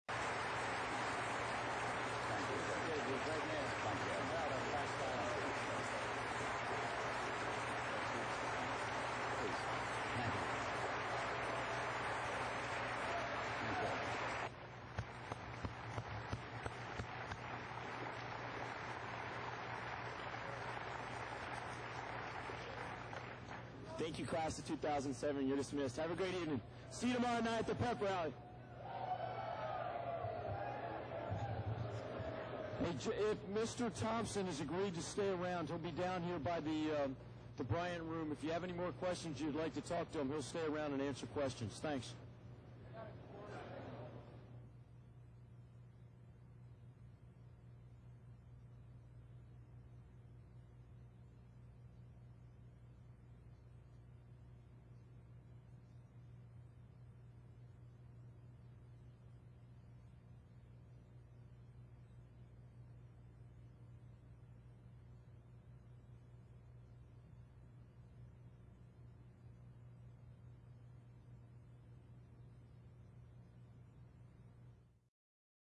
Moral Courage in Combat: The My Lai Story - Hugh Thompson Part 5
Moral Courage Lecture Series